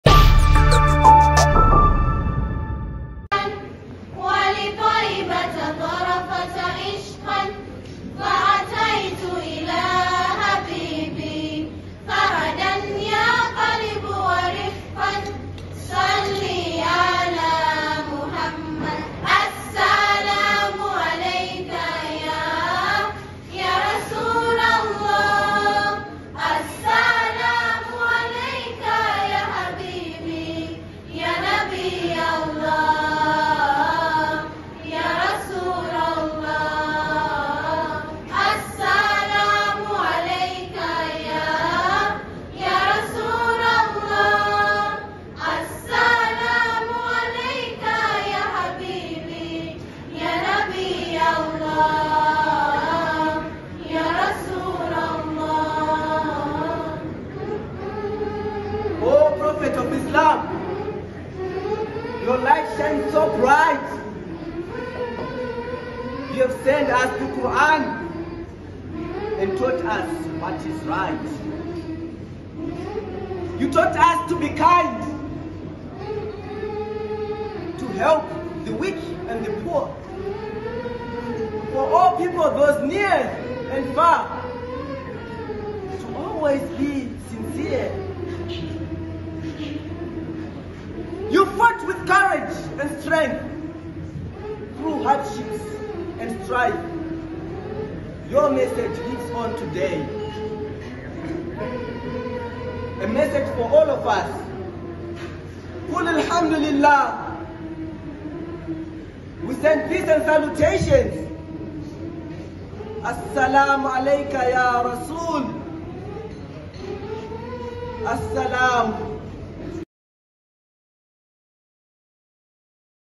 Fatima Zahra leaners perform during the 45th-anniversary celebrations of Iran
Fatima Zahra leaners perform during the 45th-anniversary celebrations of the Islamic Republic of Iran in Harare on Thursday